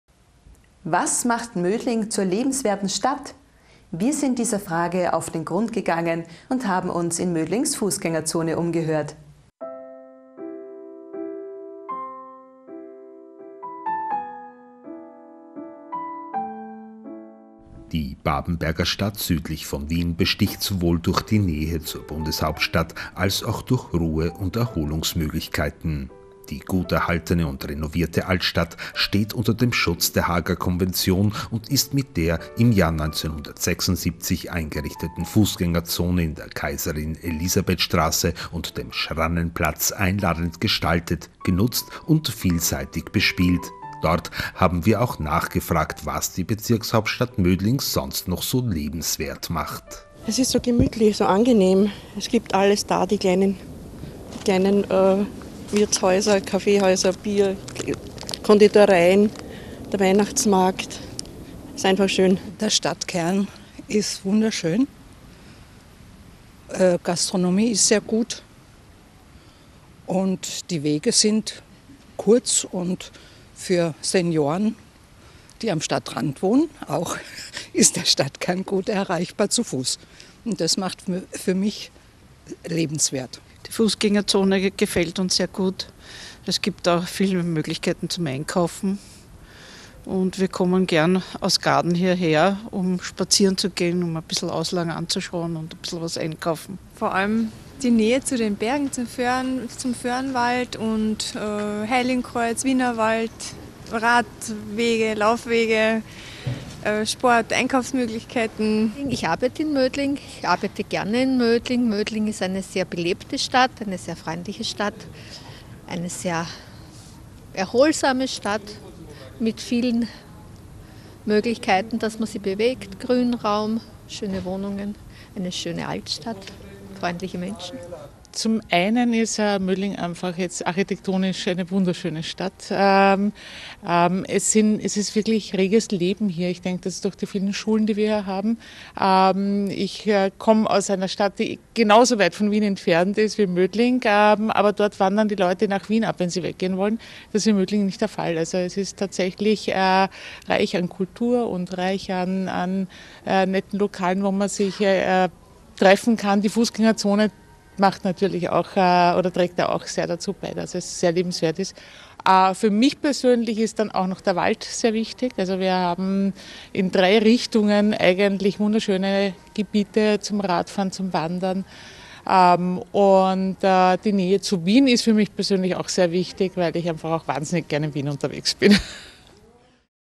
Umfrage ‚Was macht Mödling so lebenswert?‘
Was macht Mödling zur lebenswerten Stadt? Wir sind dieser Frage auf den Grund gegangen und haben uns in Mödlings Fußgängerzone umgehört.